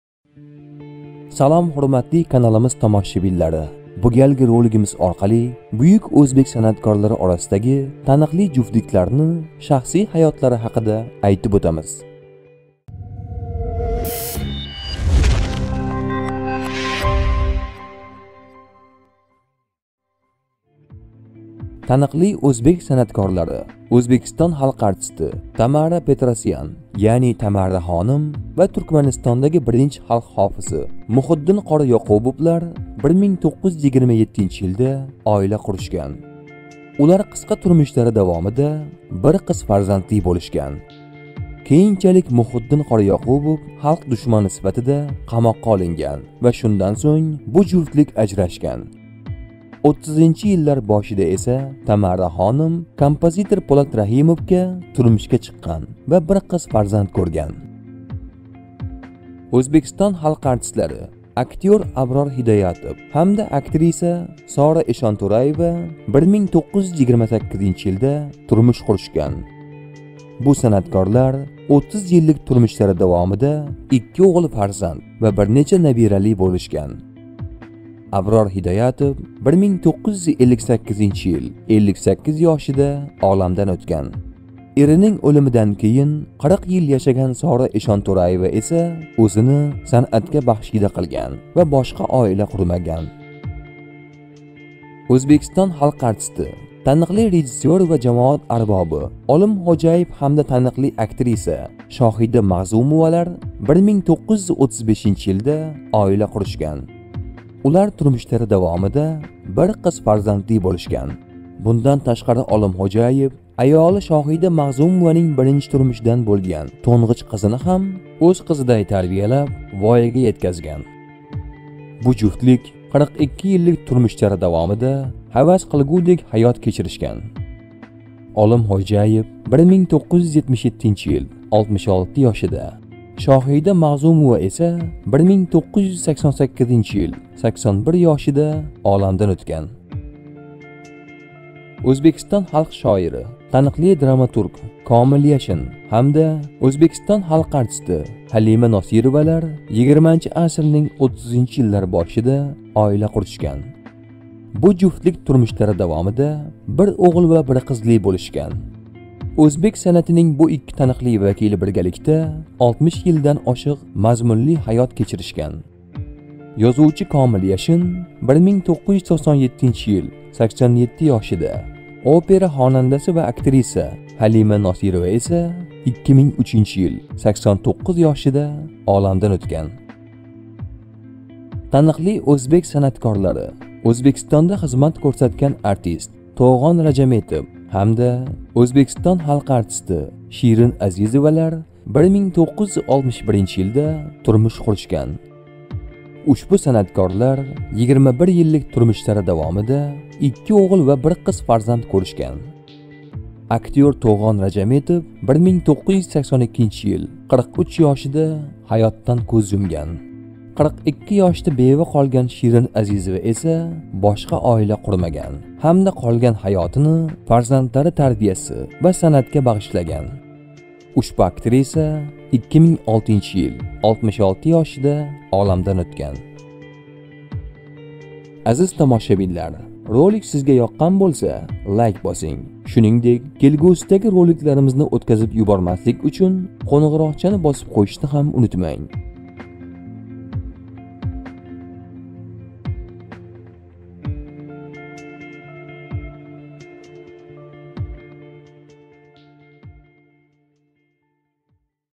Буюк Узбек санъаткорлари орасидаги таникли жуфтликлар
Узбек халкининг буюк санъаткорлари орасидаги таникли жуфтликлар хаёти хакидаги кизикарли маълумотлар